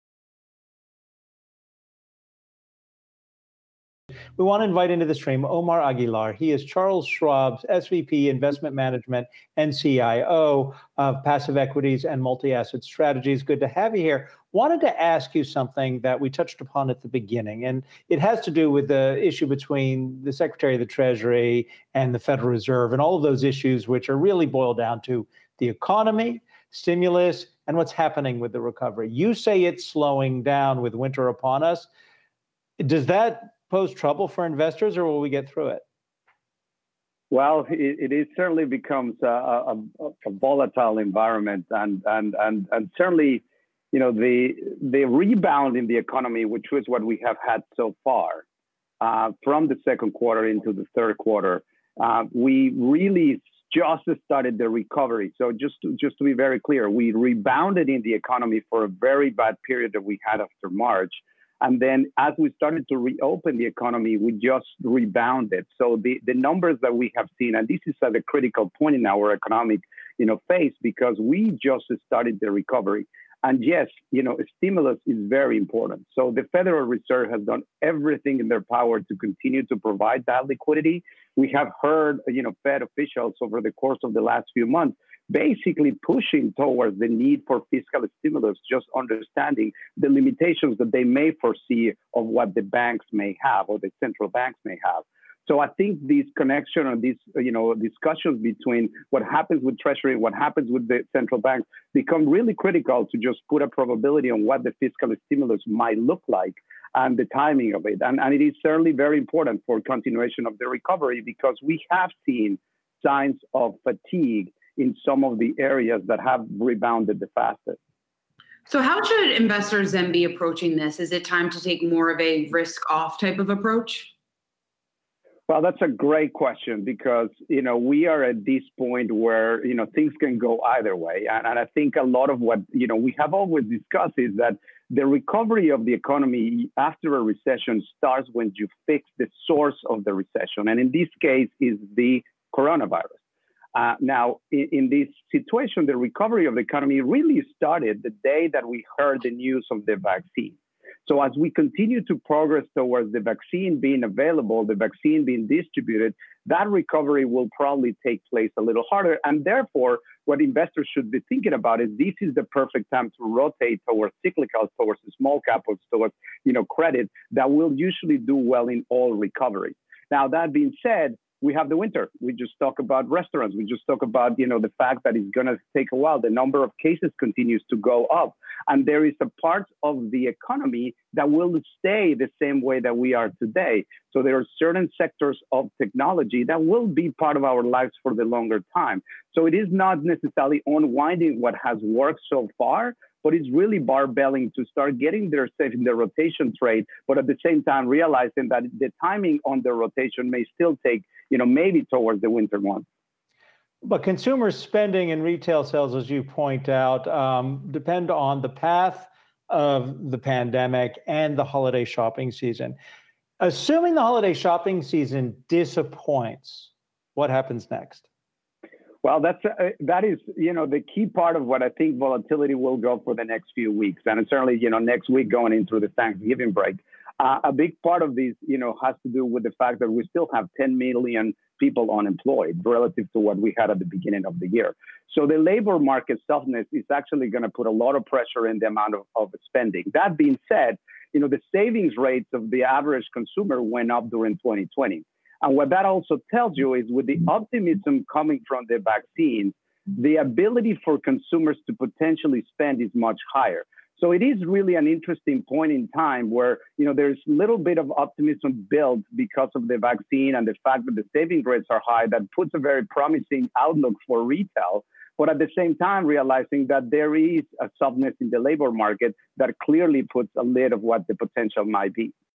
Stimulus is very important for the continuation of recovery: Strategist
Charles Schwab Investment Management SVP and CIO Omar Aguilar discusses the the outlook for the stock market, the need for stimulus amid the pandemic, and the path to economic recovery.